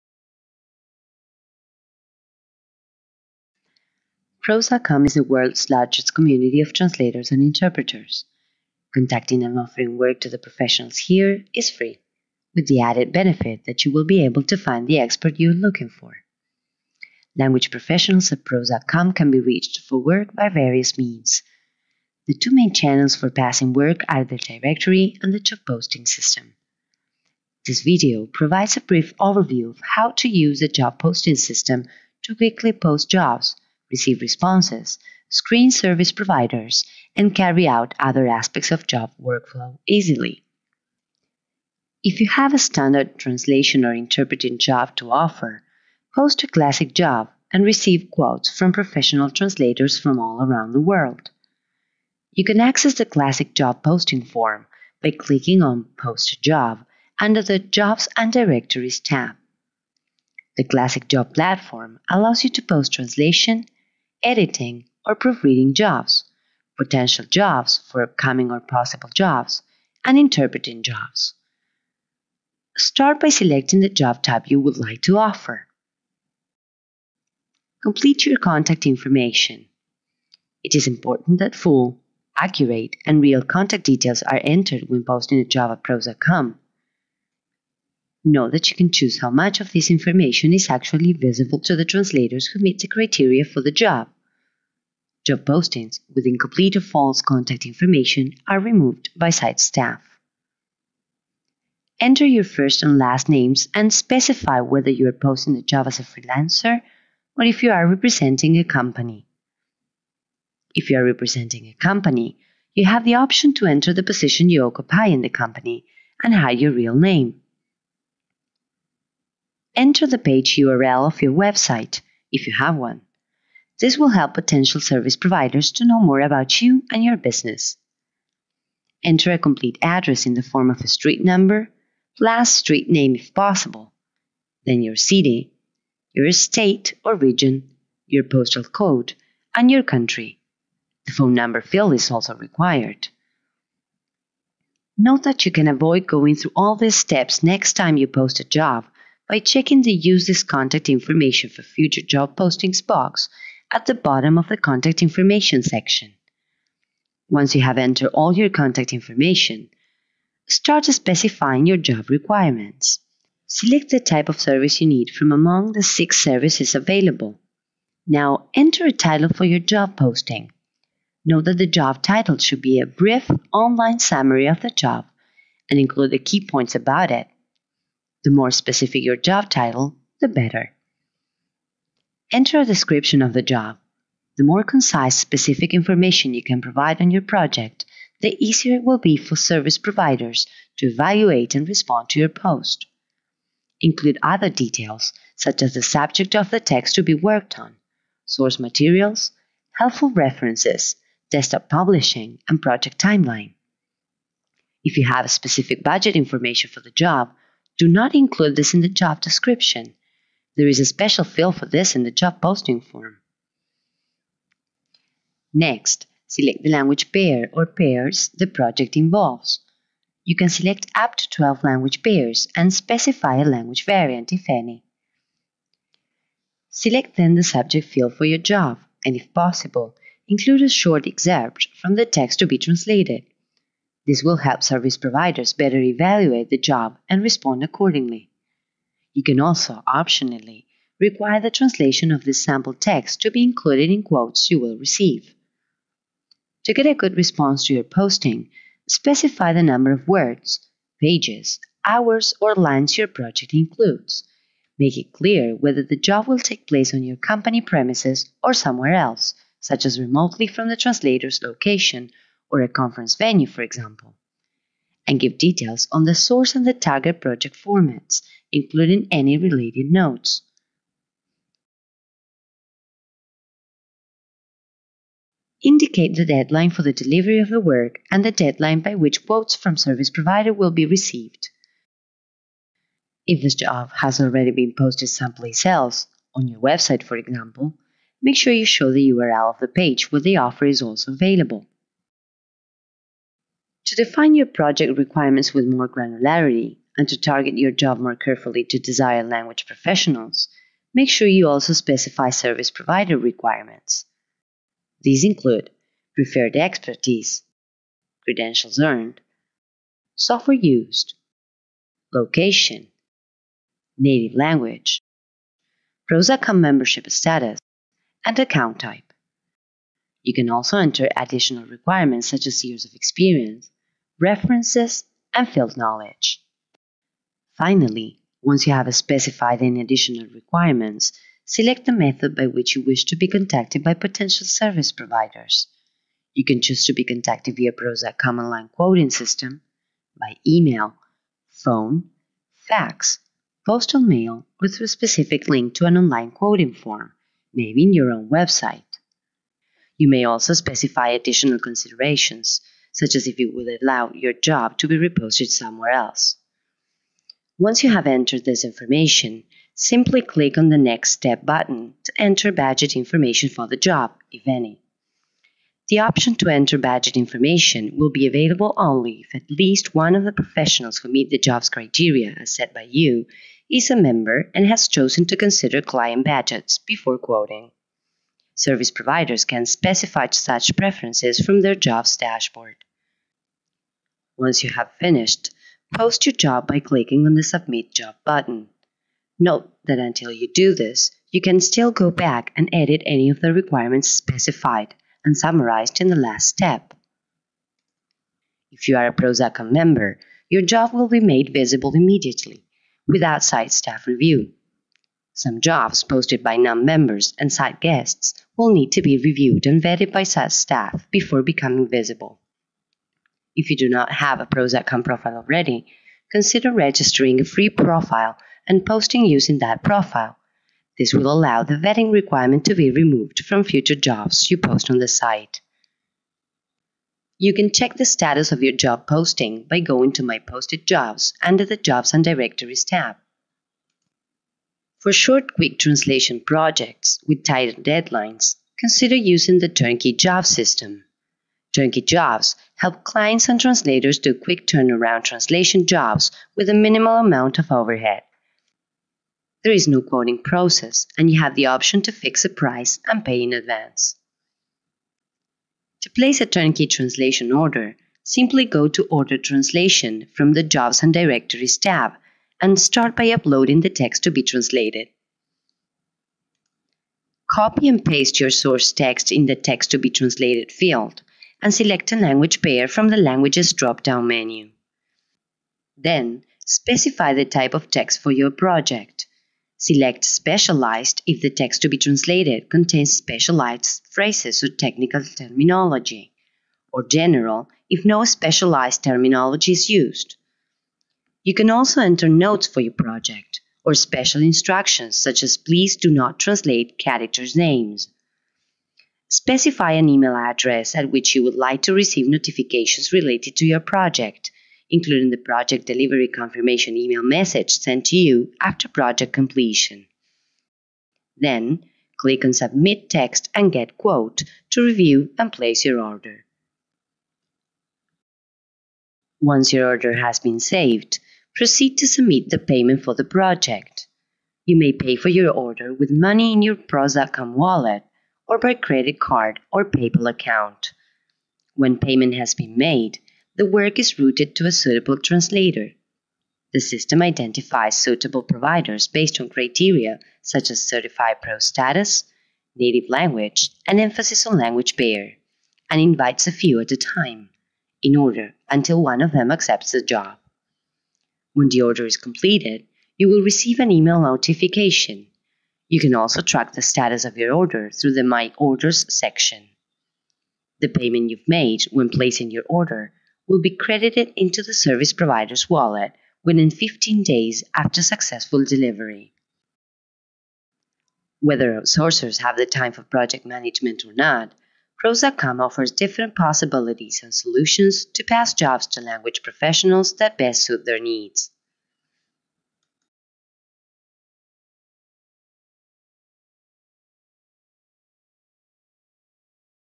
Posting a job using the ProZ.com job system
A short tutorial on using the ProZ.com job system to contact translators and interpreters for language work.